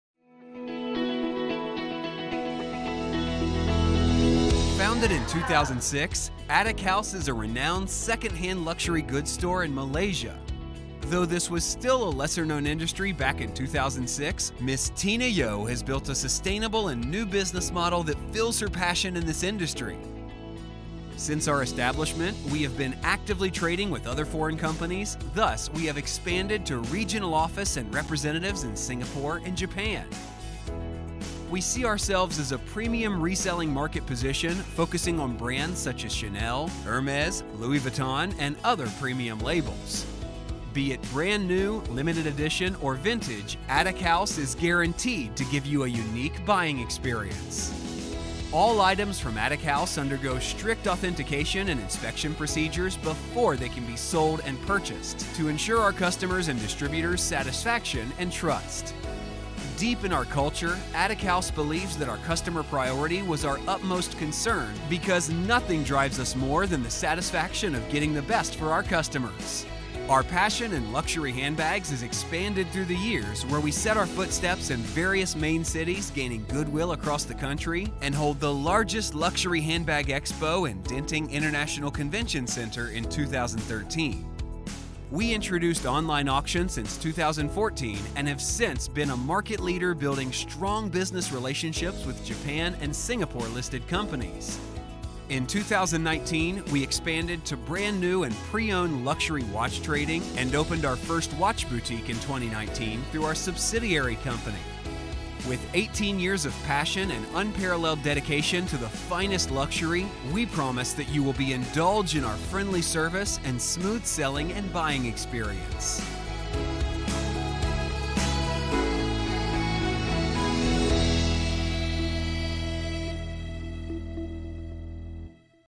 Attic House Introduction
ATTIC HOUSE was founded by Ms Tina Yow in year 2006, among the very pioneer who has started trading second hand luxury goods in Malaysia. While little is known to this industry, she has build a sustainable and profitable business that fills her passion in this industry.

Since our interception, we have been involving to deal with premium brands and exotic collections, this path a way for our future development that strengthen our knowledge and passion into vintage and rare luxury goods.